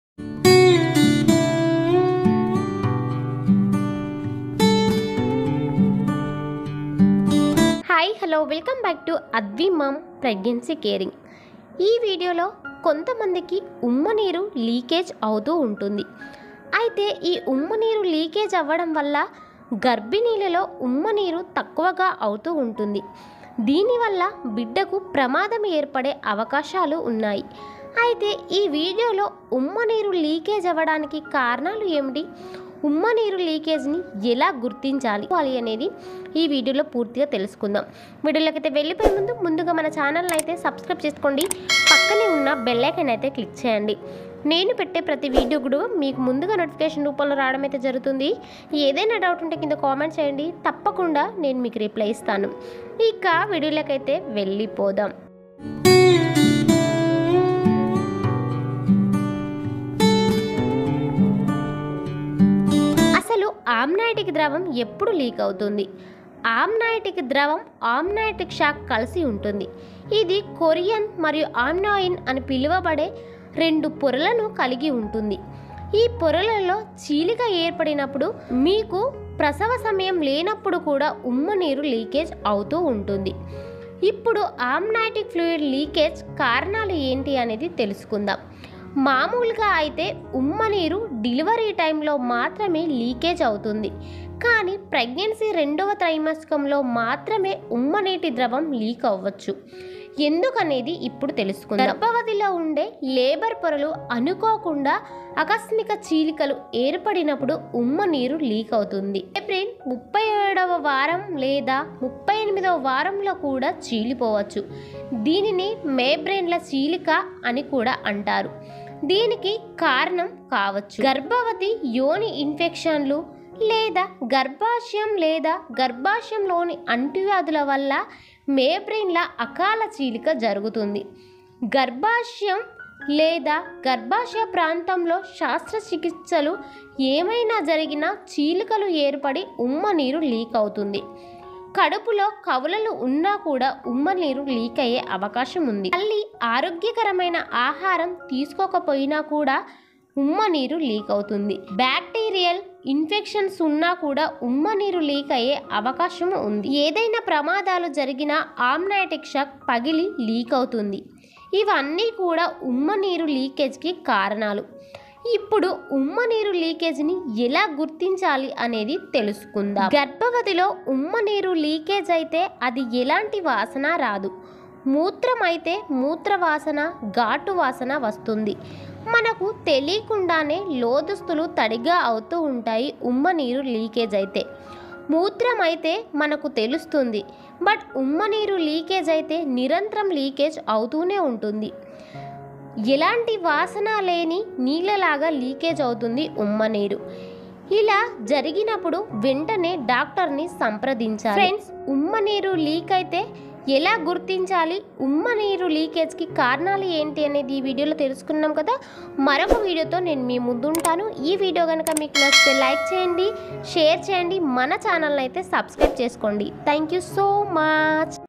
"ఉమ్మనీరు" లీకేజ్ కి కారణాలు ఏమిటి? "ఉమ్మనీరు" లీకేజ్ ని ఎలా గుర్తించాలి?@Adhvimompregnancycaring41
amnioticfluidleakagesymptoms amnioticfluidleakage amnioticfluidleakage solutions.

6. ఉమ్మనీరు తగ్గితే తల్లి శరీరంలో ఎలాంటి మార్పులు సంభవిస్తాయి
1.
yourtube channel for baby caring by advi mom